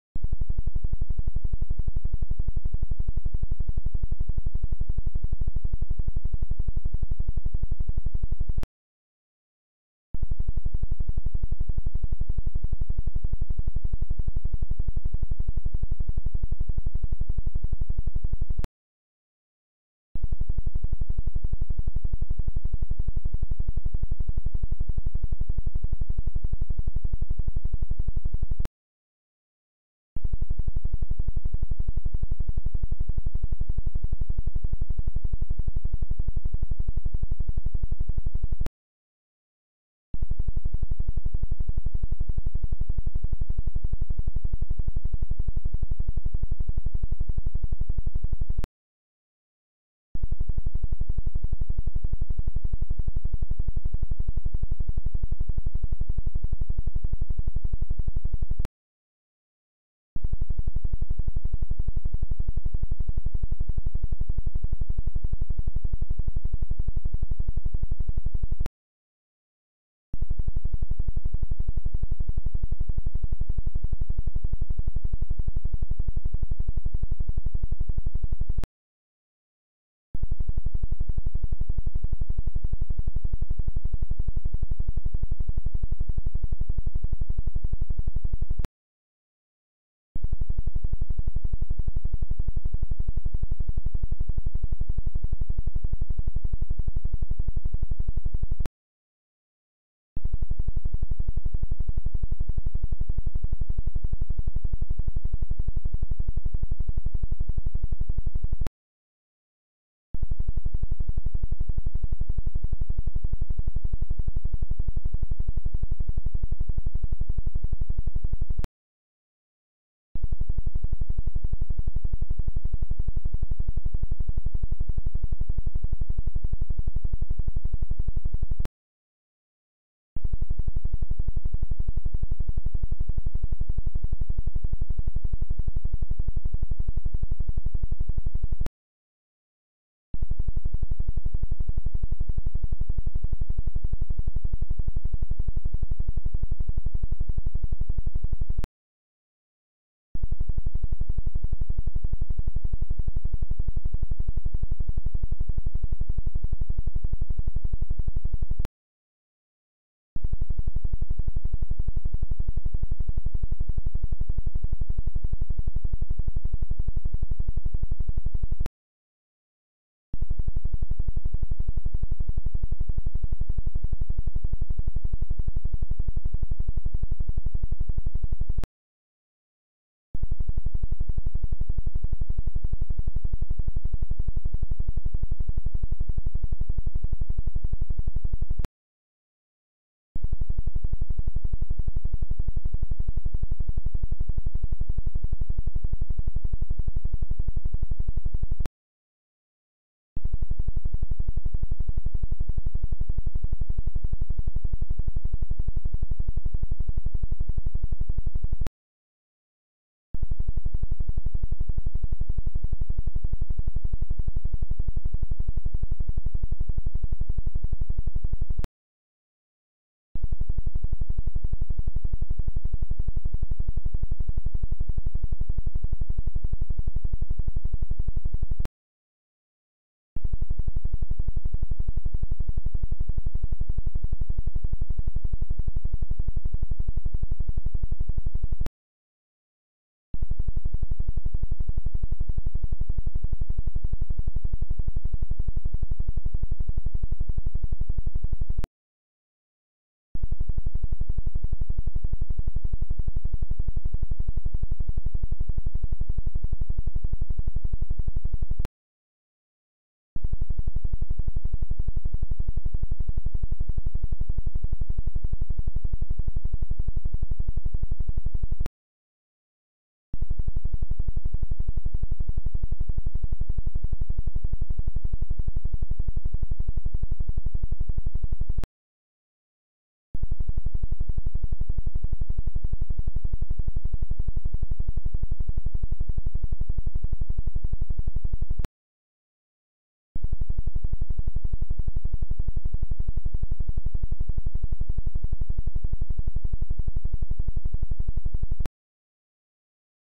Liberación de Miedos Subconscientes
LAS FRECUENCIAS  UTILIZADAS COMO TERAPIA DE ONDAS SONORAS
Como promedio las terapias deberian de ser de 20 a 30 minutos diarios.
Puede ser escuchado con o sin auriculares.

Eternale Biofrecuencias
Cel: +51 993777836